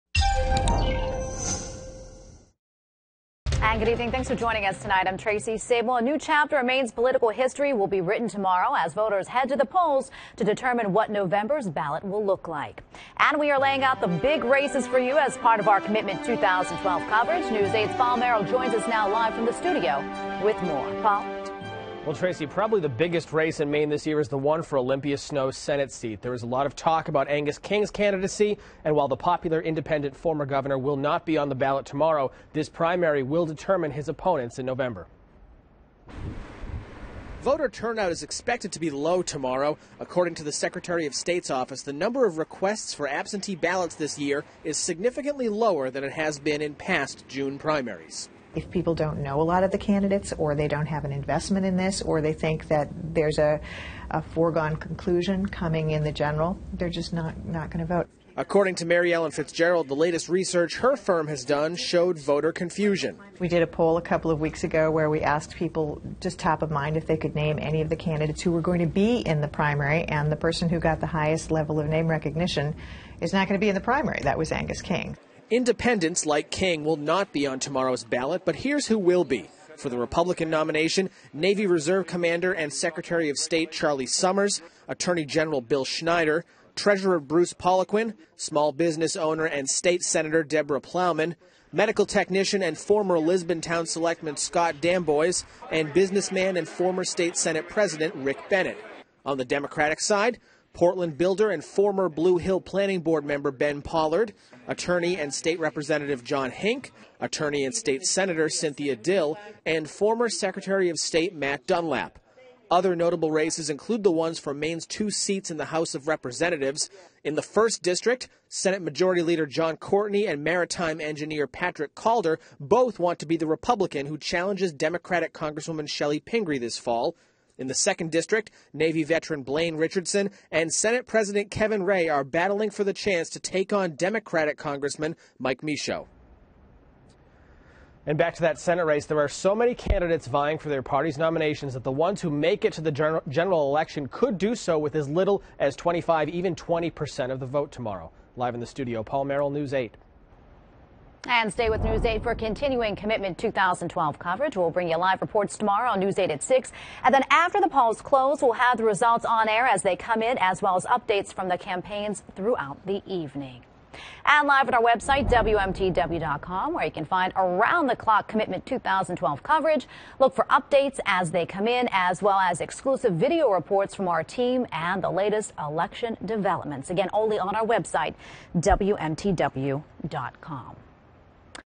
Candidates are gearing up for Tuesday's primary
One of the biggest races on the primary ballot on Tuesday is the one for Olympia Snowe's senate seat.  News 8's Paul Merrill reports.